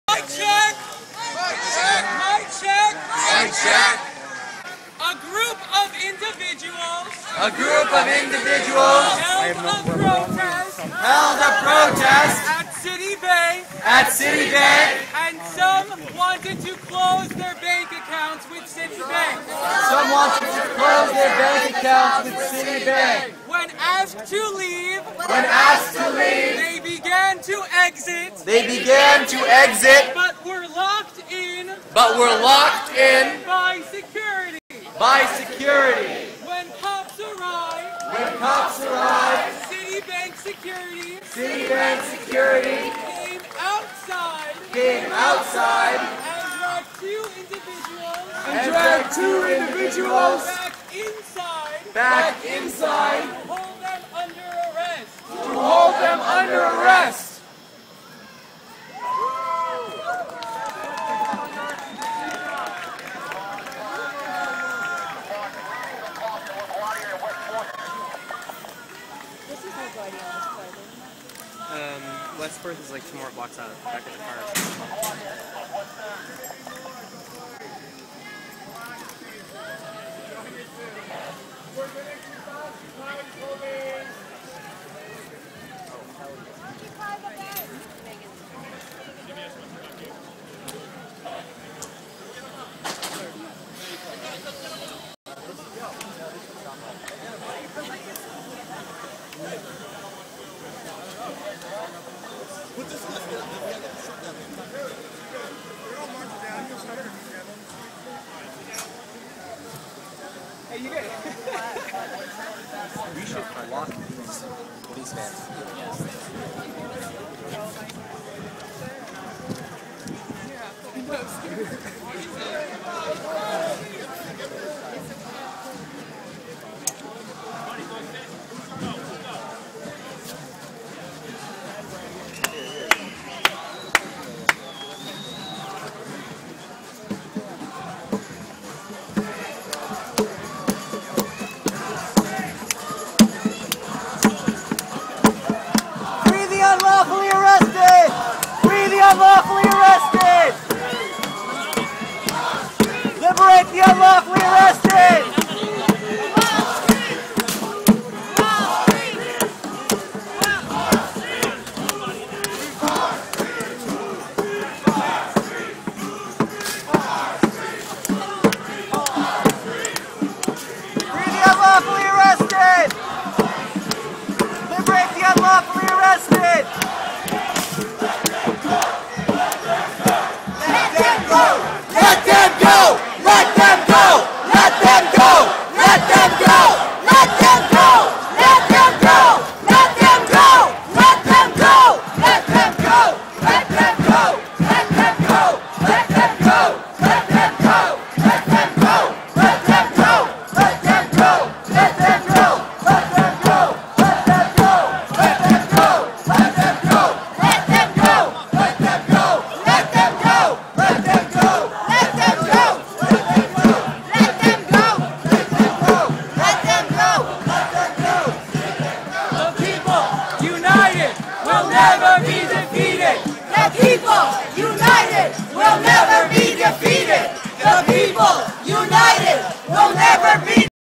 Video of arrests at citibank near WA Sq NY. O15 occupyWallSt OWS occupyamerica.ning.com.mp4
Video of arrests at citibank near WA Sq NY.